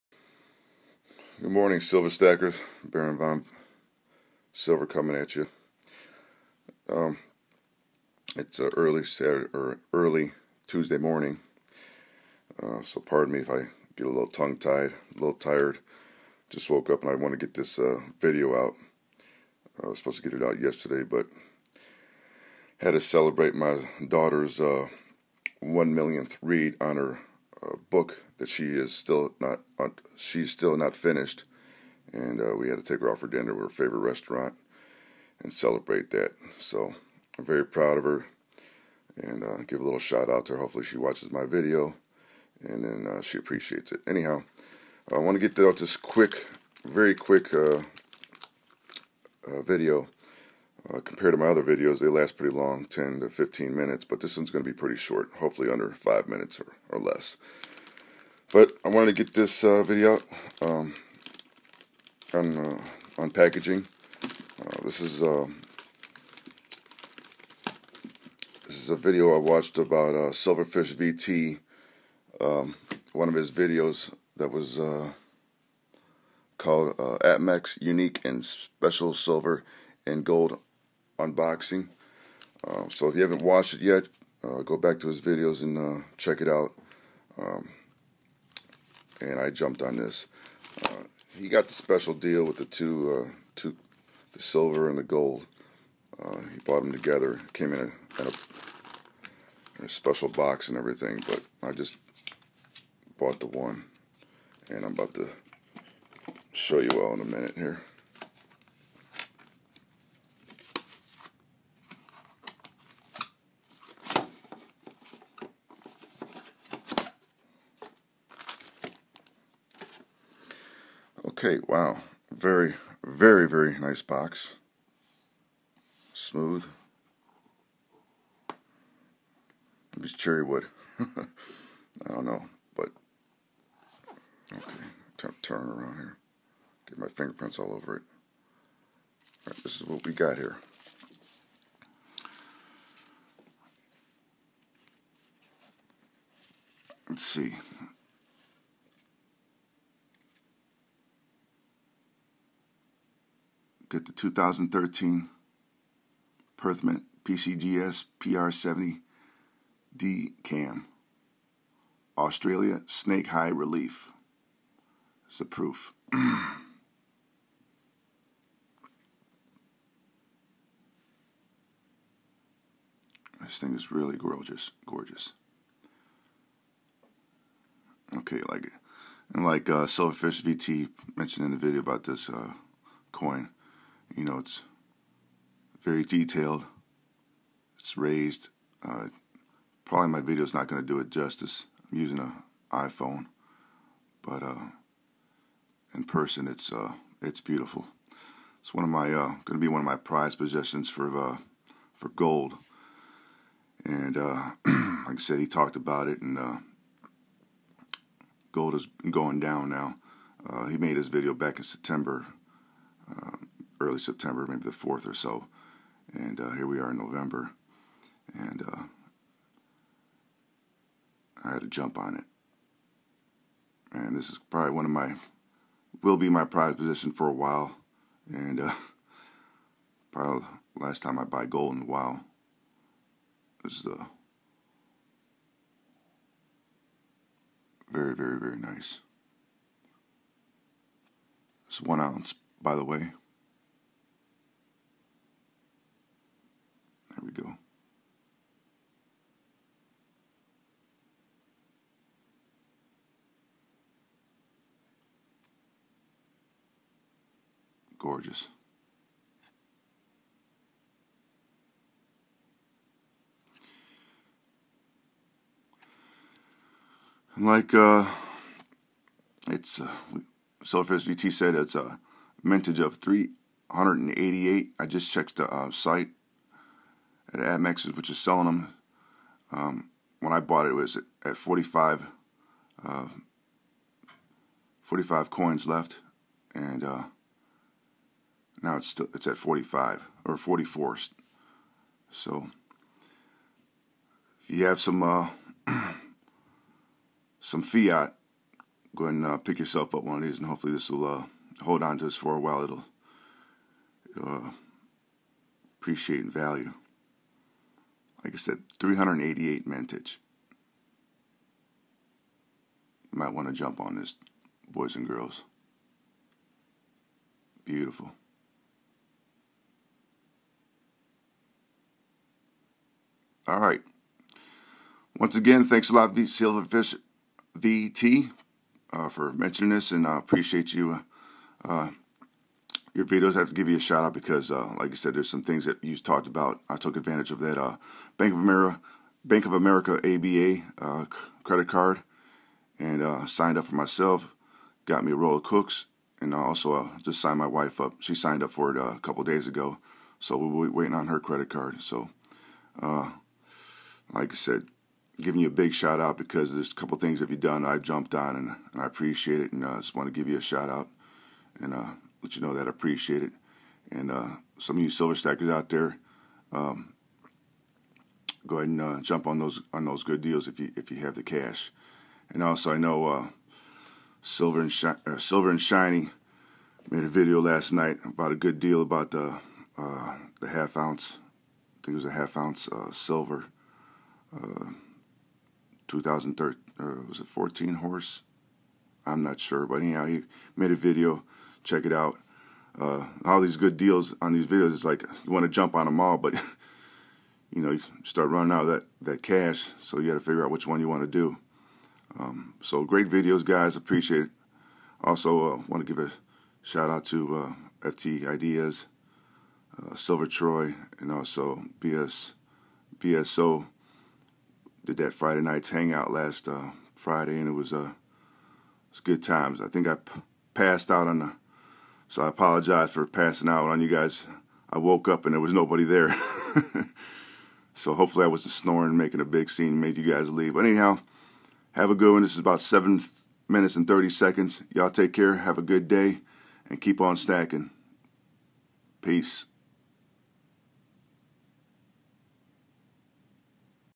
2013 1 oz Ultra High Relief Proof Gold Snake PCGS PR-70 DCAM YUMMY!!!!
I had to jump on this deal thanks to Silverfish VT! 388 Mintage! Beautiful coin! Keep stacking, HEAVY! The mint # on this coin is 253 of 388! P.S. Don't do videos when just waking up in the morning. Free advice! lol!